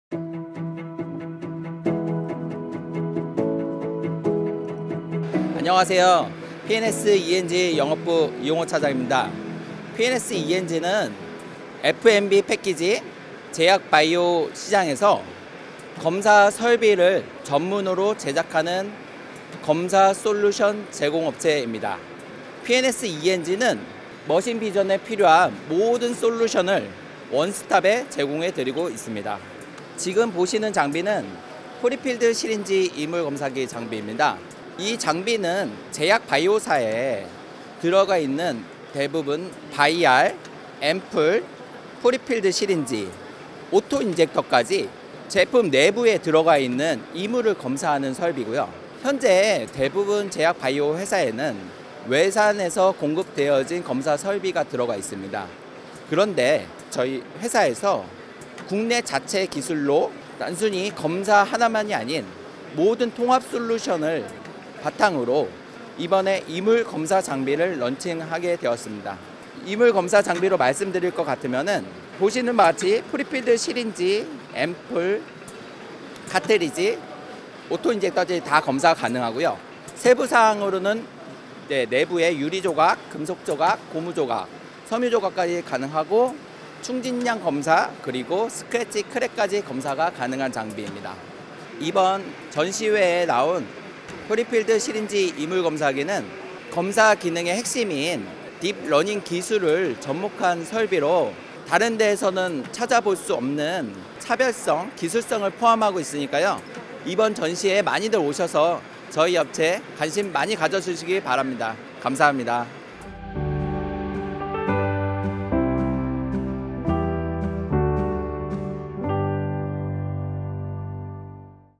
[SFAW2023] 피엔에스이엔지의 프린필드 시린지 이물 검사기를 소개합니다
피엔에스이엔지의 프린필드 시린지 이물 검사기 및 주요 제품을 확인하세요

Asia No.1 자동화 전시회!
산업 지능화 시대를 선도하는 아시아 최고의 스마트공장•자동화산업전 현장을 살펴봅니다
■ Smart Factory+Automation World 2023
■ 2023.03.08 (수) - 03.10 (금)
■ COEX A,B,C,D Hall